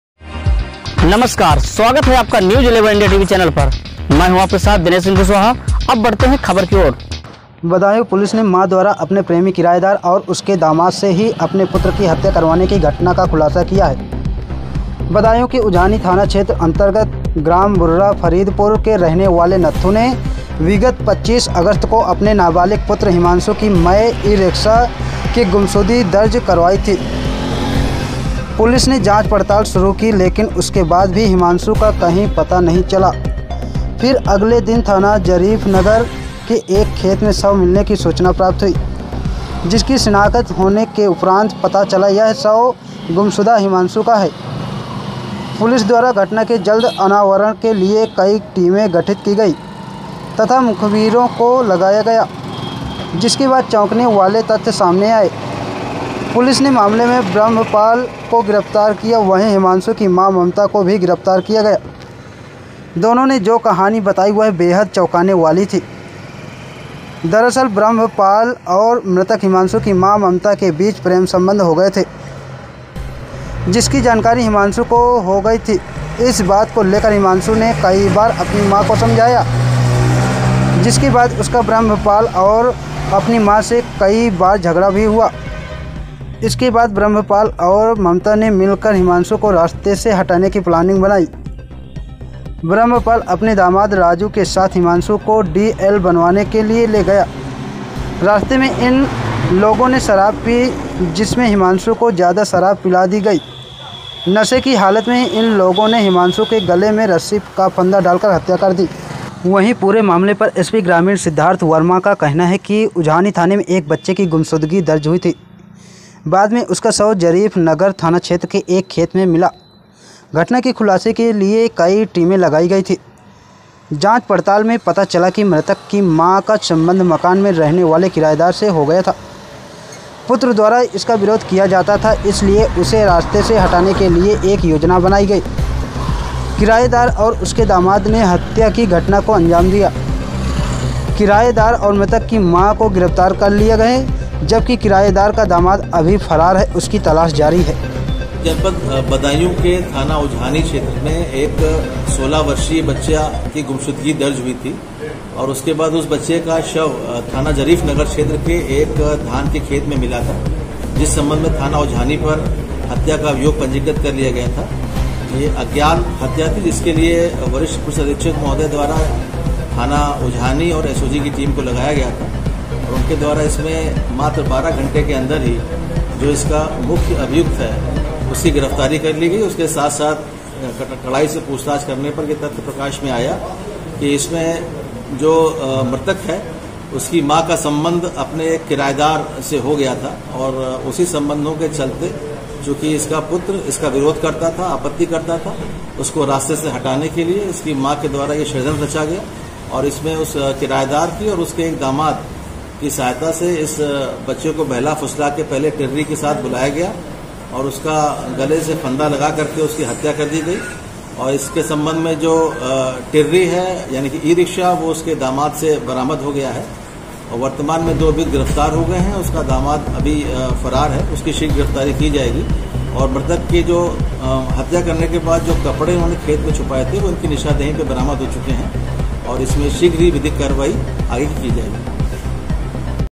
बदायूँ पुलिस ने माँ द्वारा अपने प्रेमी किरायेदार,उसके दामाद से ही अपने पुत्र की हत्या करवाने की घटना
बदायूं के उझानी थाना क्षेत्र अंतर्गत ग्राम बुर्रा फरीदपुर के रहने वाले नत्थू ने विगत 25 अगस्त को अपने नाबालिग पुत्र हिमांशु की मय ई रिक्शा के गुमशुदगी दर्ज करवाई थी। पुलिस ने जांच पड़ताल शुरू की लेकिन उसके बाद भी हिमांशु का कहीं पता नहीं चला फिर अगले दिन थाना जरीफनगर के एक खेत में शव मिलने की सूचना प्राप्त हुई जिसकी शिनाख्त होने के उपरांत पता चला यह शव गुमशुदा हिमांशु का है। पुलिस द्वारा घटना के जल्द अनावरण के लिए कई टीमें गठित की गई तथा मुखवीरों को लगाया गया जिसके बाद चौंकने वाले तथ्य सामने आए। पुलिस ने मामले में ब्रह्मपाल को गिरफ्तार किया वही हिमांशु की मां ममता को भी गिरफ्तार किया गया, दोनों ने जो कहानी बताई वह बेहद चौंकाने वाली थी। दरअसल ब्रह्मपाल और मृतक हिमांशु की मां ममता के बीच प्रेम संबंध हो गए थे जिसकी जानकारी हिमांशु को हो गई थी इस बात को लेकर हिमांशु ने कई बार अपनी मां को समझाया, जिसके बाद उसका ब्रह्पाल और अपनी मां से कई बार झगड़ा भी हुआ। इसके बाद ब्रह्मपाल और